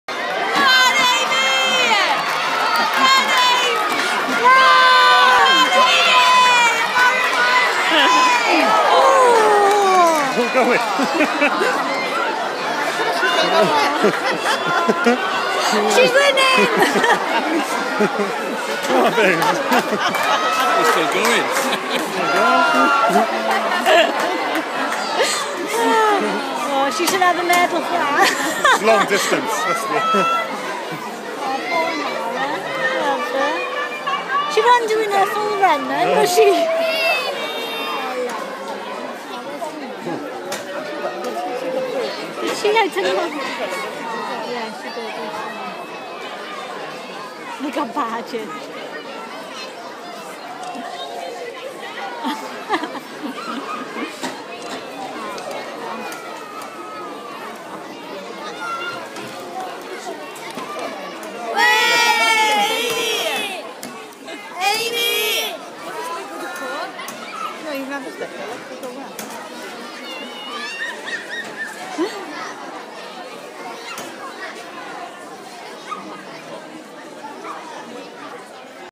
100m School Sports Day
Just like her dad not the best off the mark but check her go over the 100m there was no stopping her. That's what we had been training for